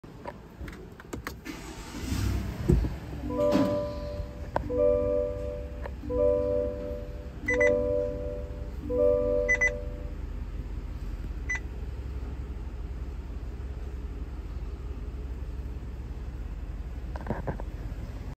Reset Your 2024 Cadillac Escalade Oil Life Percentage
Complete this process after changing the oil and oil filter on your Cadillac Escalade. This procedure will allow you to complete a full maintenance light reset for the next scheduled oil change. This process will reset the time and distance until your next service.